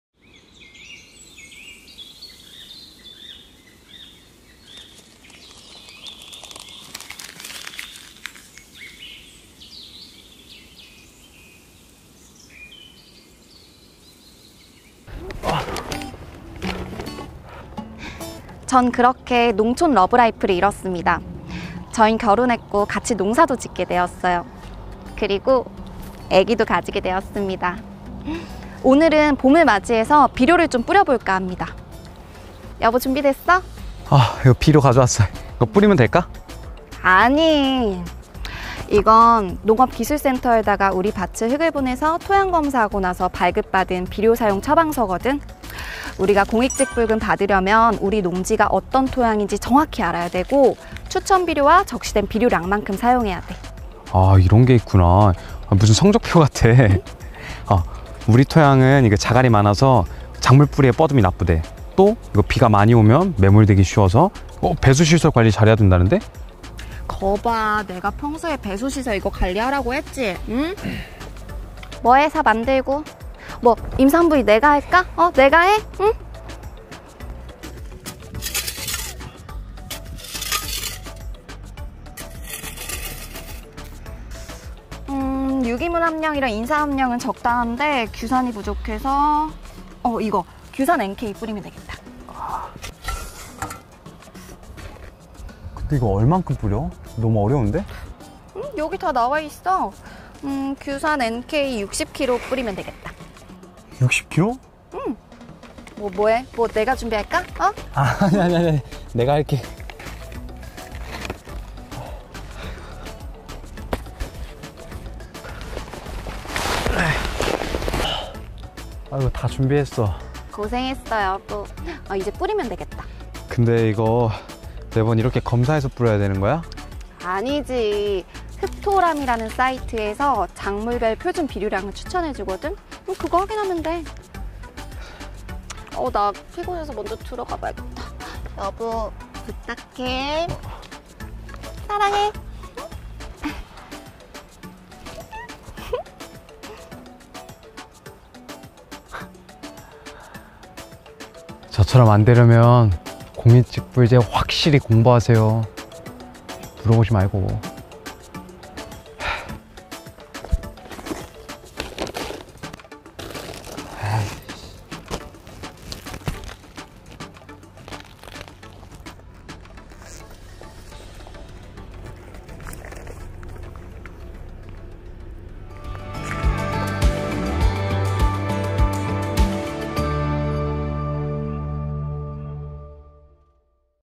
공익직불 의무준수사항 4편 화학비료사용 기준 준수!
✔️ ph, 유기물함량, 유기인산함량, 교환성칼륨함량 점검

🏁 국립농산물품질관리원